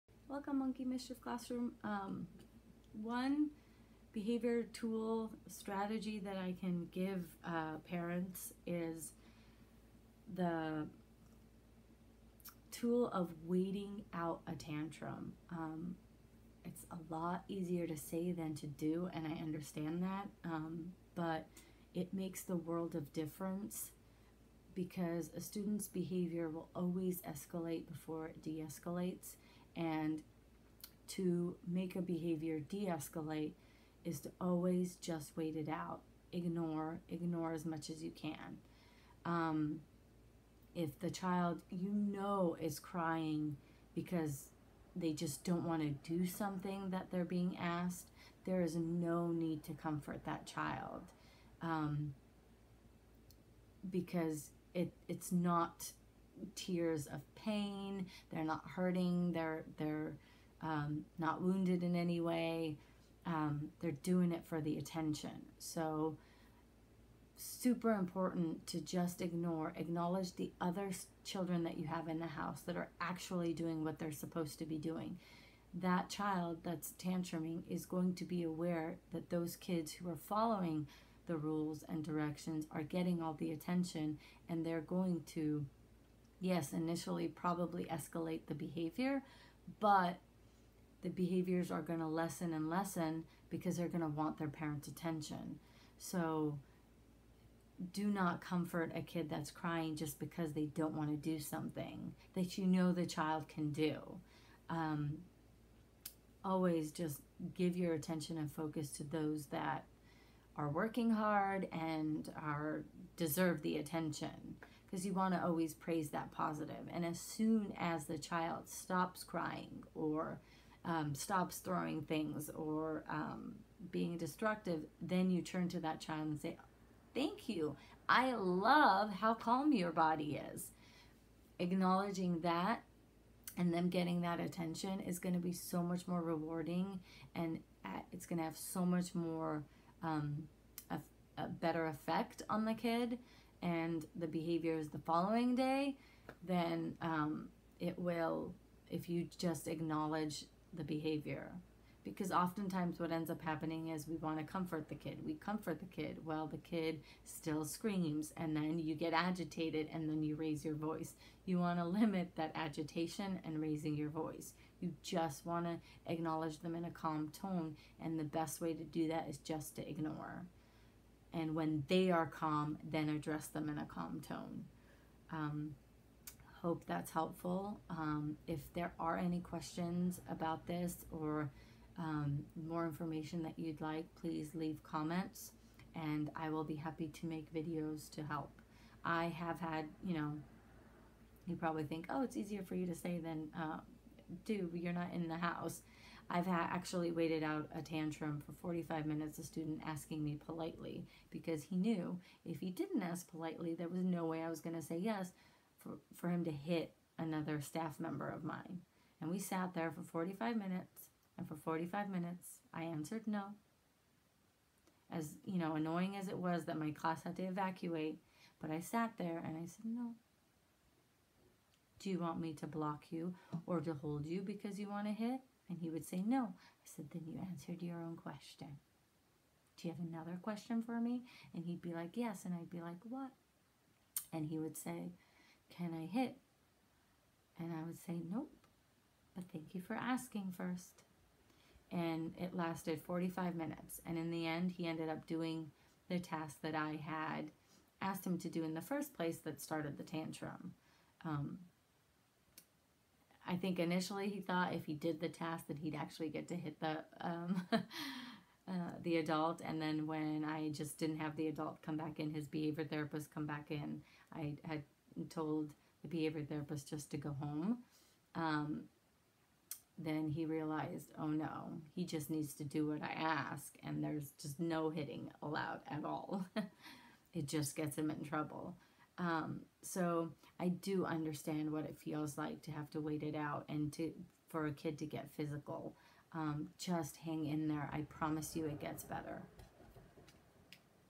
Waiting out a Tantrum
Tantrums are probably the most difficult part of working with students with behaviors.  The quicker you learn to ignore the behaviors the faster the behavior will subside.